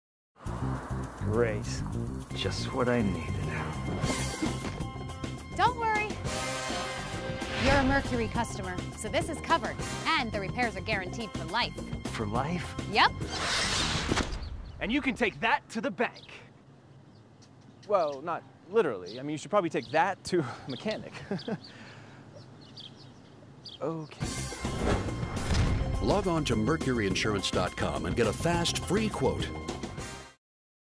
Mercury Mission Guaranteed Repairs
Mercury Insurance commercial starring Heather Storm.